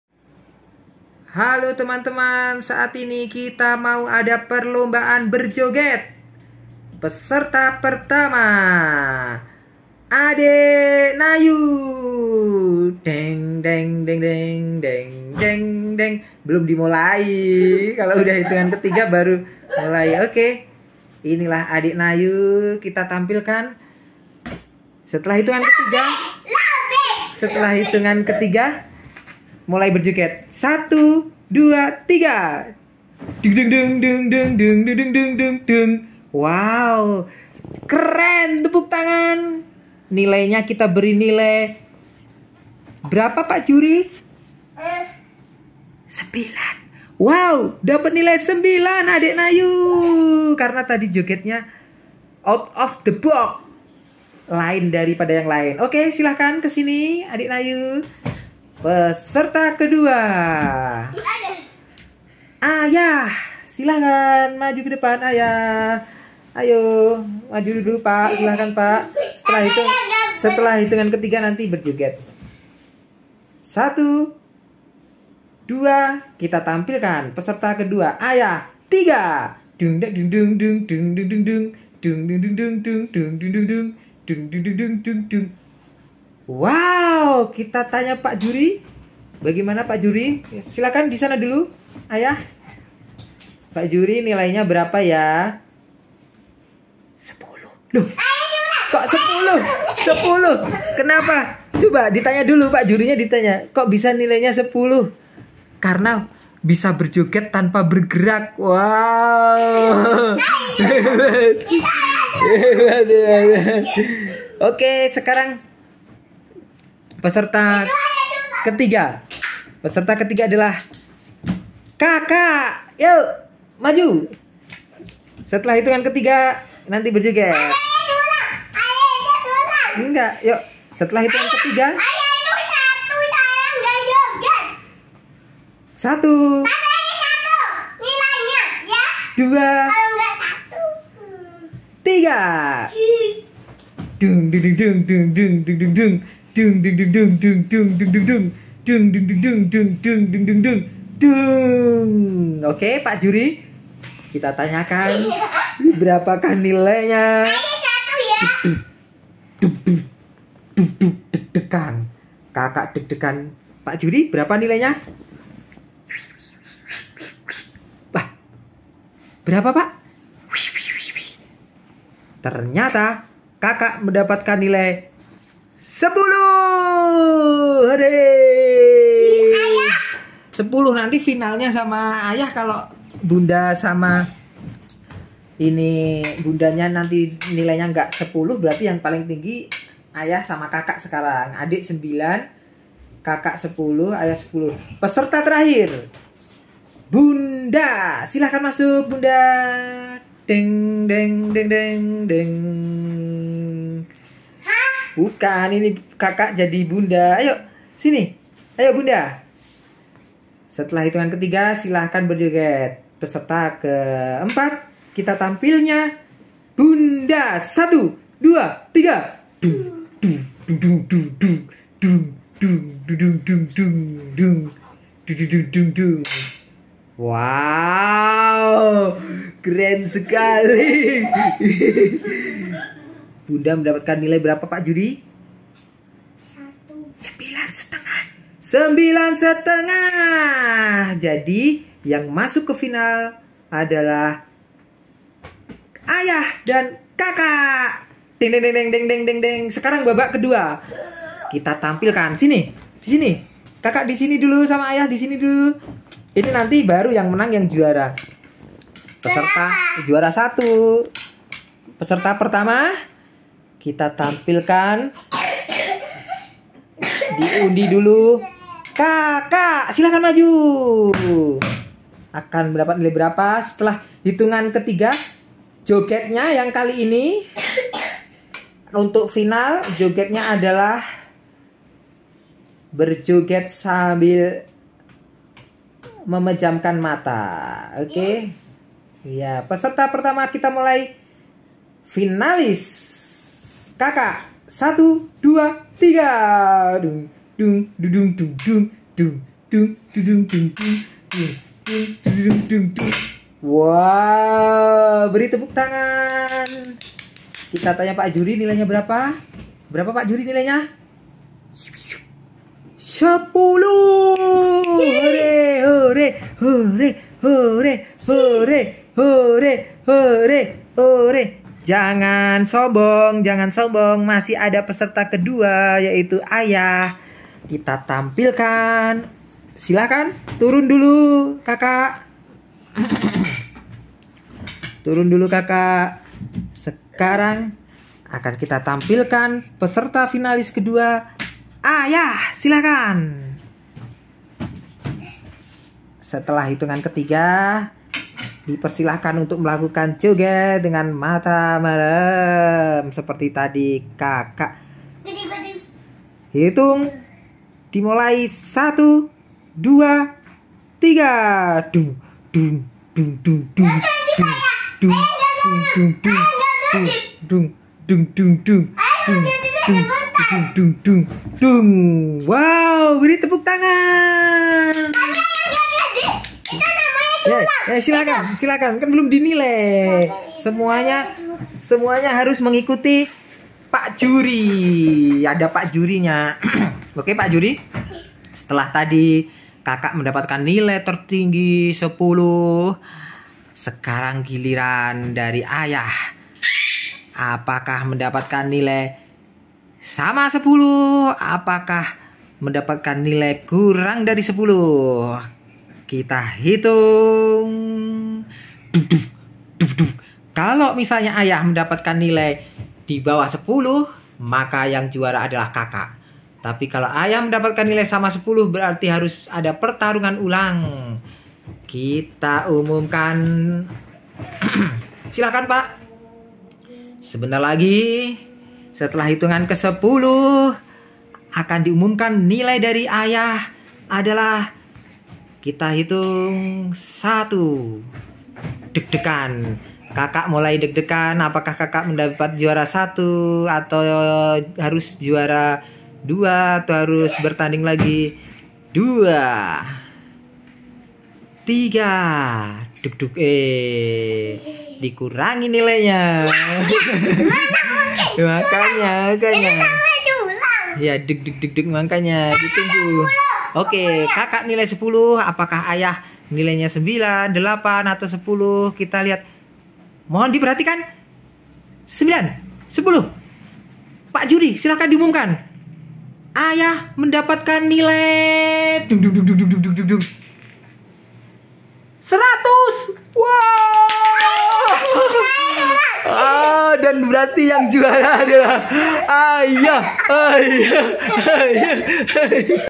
ADEK, KAKAK, AYAH, BUNDA IKUT LOMBA JOGET.. YANG JUARA 1 ADALAH.. SIAPA HAYOOO? COBA TEBAK!
LOMBA JOGET MENARI